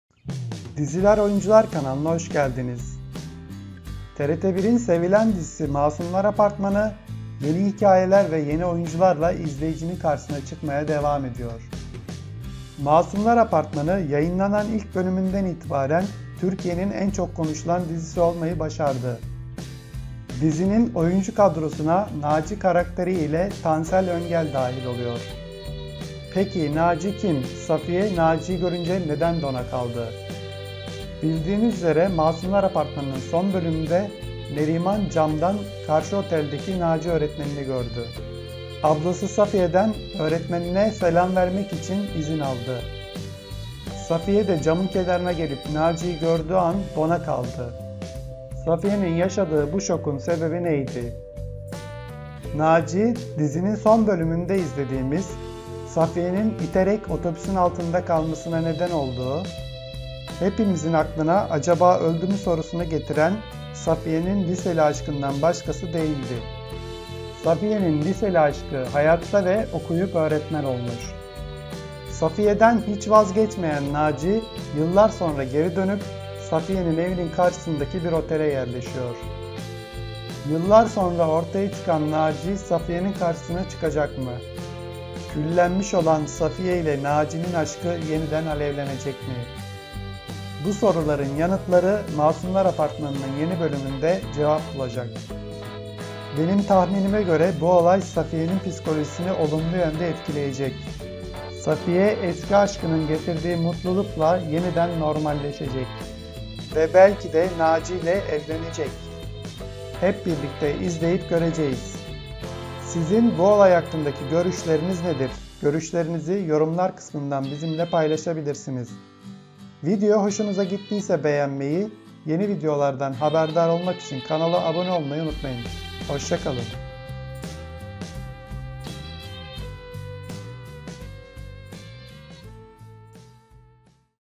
Masumlar Apartmanı Safiye'nin Sevgilisi Naci kim? - Tansel Öngel
Masumlar Apartmanı'nın dün akşam yayınlanan bölümünde Safiye'nin lise yıllarında ölümüne sebep olduğunu düşündüğü sevgilisi ortaya çıkıyor. Dizide Safiye'nin aşkı Naci'yi ise oyuncu Tansel Öngel oynayacak.

TRT 1’in sevilen dizisi MasumlarApartmanı yeni hikayeler ve oyuncular ile izleyicisi karşısına çıkmaya devam ediyor.

Yayınlandığı günden itibaren Türkiye’nin en çok konuşulan dizilerinden olan Masumlar Apartmanı dizisi oyuncu kadrosuna Naci karakteri ile TanselÖngel dahil OLUYOR.

 Naci oldukça farklı bir adamdır, şiiri sanatı seven Naci hoca edebiyat öğretmenidir. Aynı zamanda çok güzel klarnet de çalmaktadır.

Naci Safiye’nin yıllar önce aşık olduğu ancak psikolojik sorunları nedeniyle kalbini kırdığı ve kaza yapıp yaralanmasına sebep olduğu eski sevgilisidir. Naci şimdilerde Neriman’ın okulunda öğretmendir. Naci aradan yıllar geçmesine rağmen Safiye’den vazgeçmemiş onların penceresini gören bir ev tutmuş ve yıllar boyunca görünmeden Safiye’yi takip etmiştir.